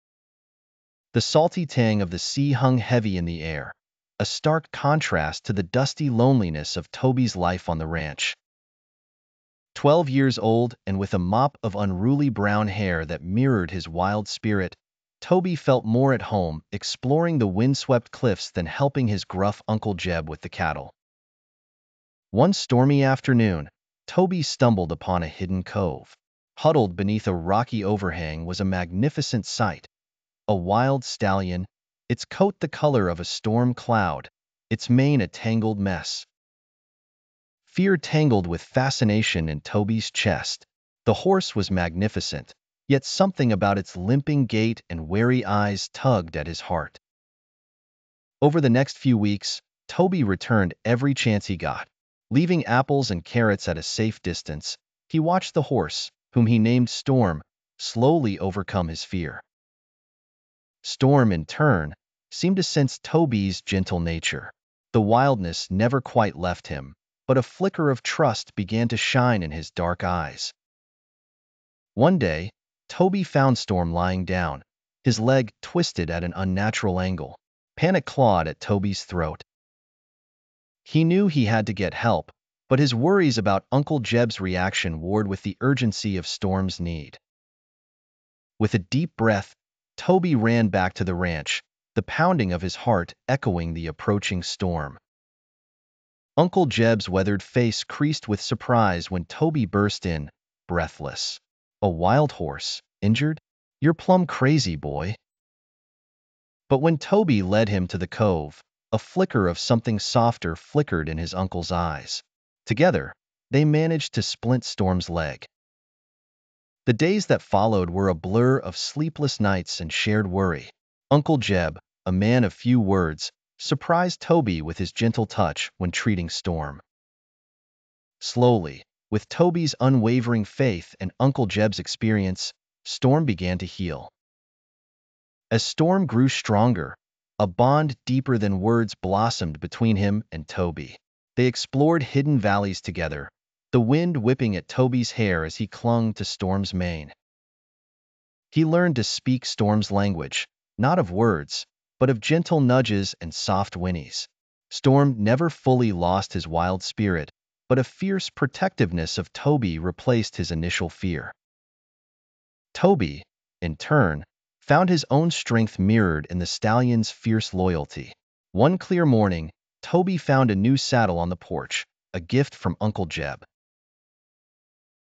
Storm a Boy and His Wild Mustang
Twelve-year-old Toby discovers a wounded wild stallion, Storm, in a hidden cove. Despite initial fear, Toby forms a deep bond with Storm, aided by his uncle. Through care and trust, Storm heals, leading to an unbreakable friendship as they explore the ranch together.